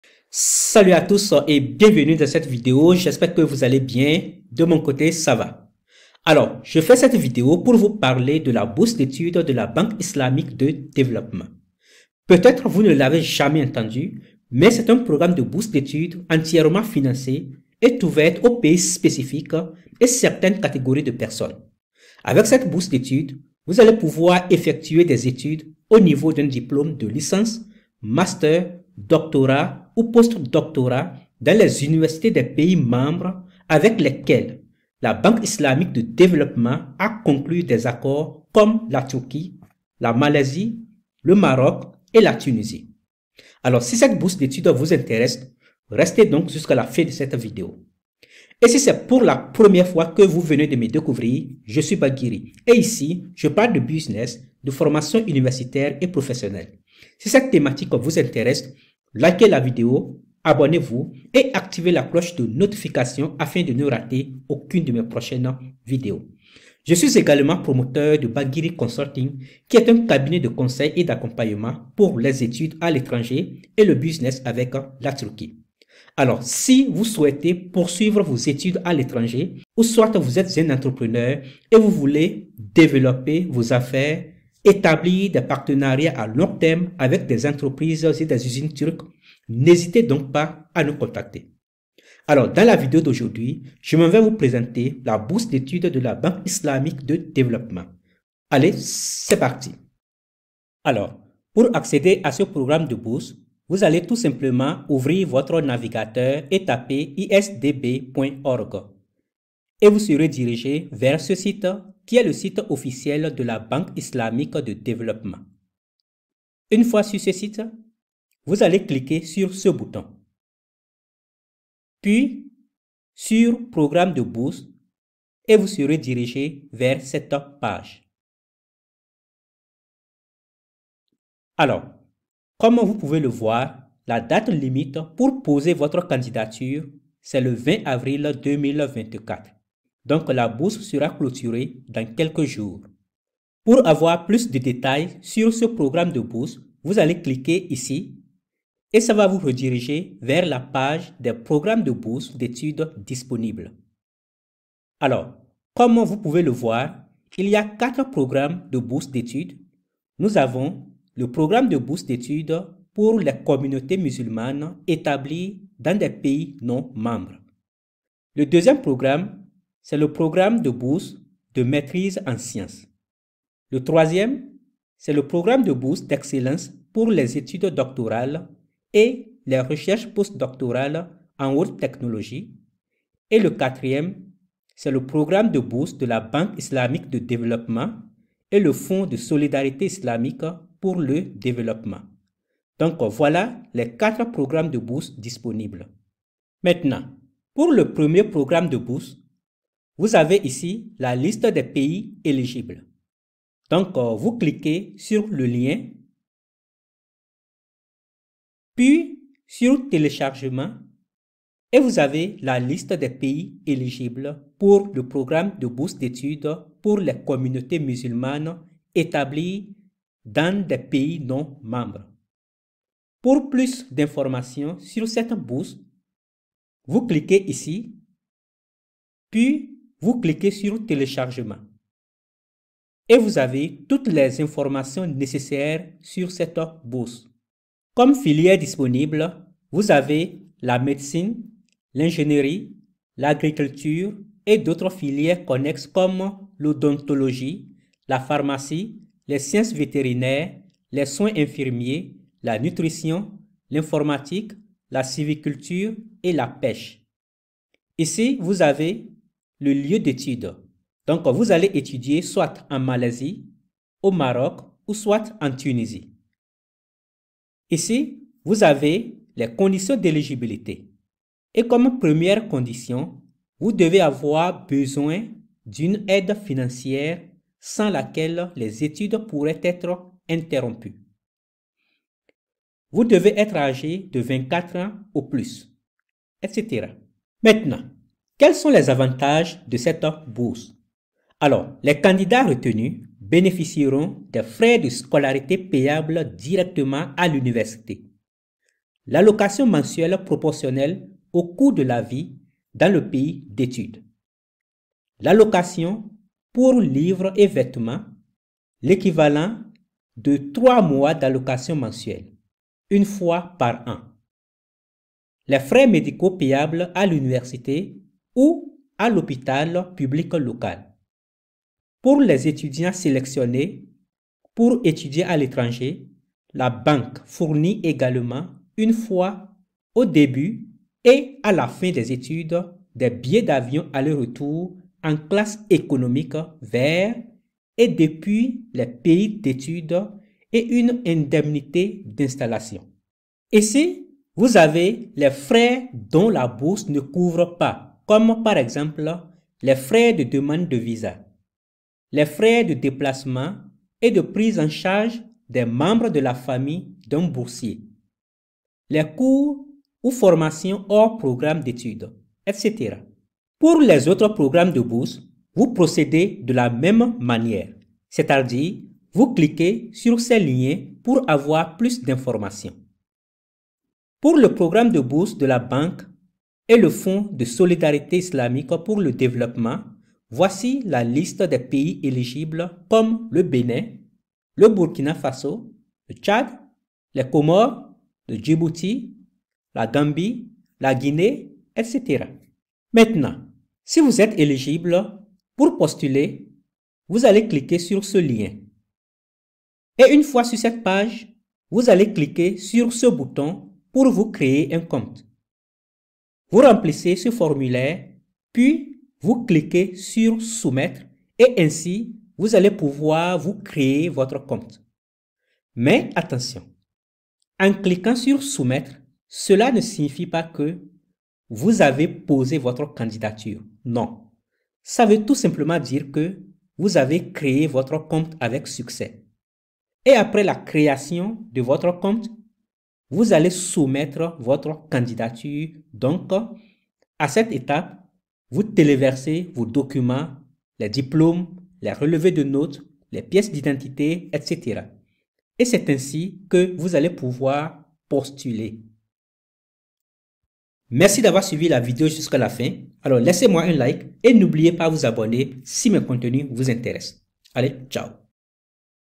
Bourses d’Étude complète pour étudier en Turquie, Malaisie, Maroc, Tunisie en 2024 - 2025
Dans cette vidéo, je vous présente le programme de la bourse d’étude de la Banque Islamique de Développement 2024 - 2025.

NB: AVIS DE NON-RESPONSABILITÉ : Cette vidéo est réalisée à des fins éducatives et/ou informatives. Veillez faire votre propre recherche et surtout consultez toujours les sites officiels. Je n'offre aucune bourse, visa ou emploi à personne.